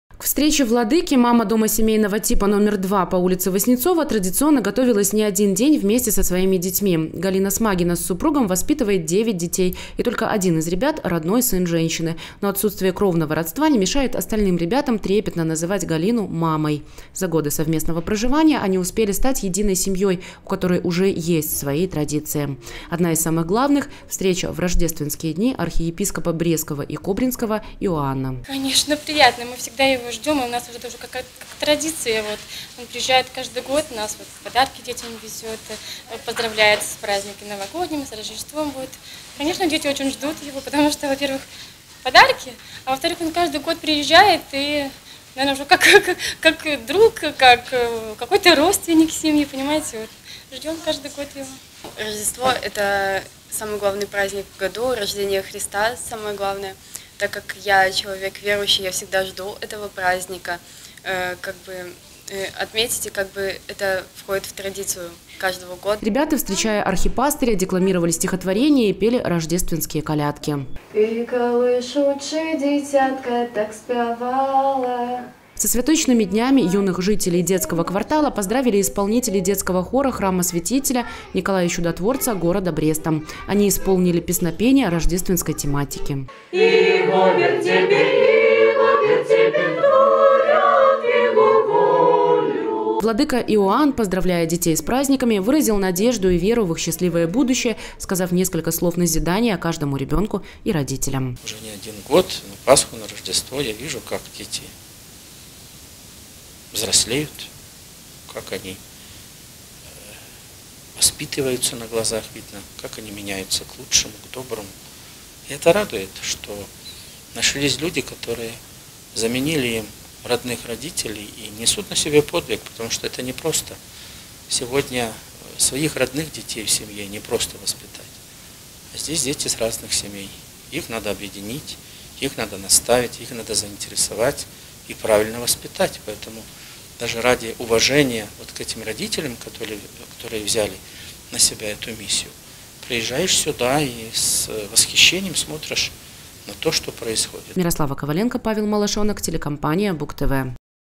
2020-01-17 г. Брест. Поздравления архиепископа в «Детском квартале». Новости на Буг-ТВ.
Журналист М. Коваленко. Оператор П. Малашенок
Поздравления архиепископа Брестского и Кобринского Иоанна в «Детском квартале» на Васнецова.
В рамках традиционных Рождественских святочных поздравлений архиепископ Брестский и Кобринский Иоанн посетил ряд домов семейного типа, расположенных в «Детском квартале» по улице Васнецова в городе Бресте. Какой получилась встреча Владыки и подрастающего поколения областного центра – расскажет Мирослава Коваленко.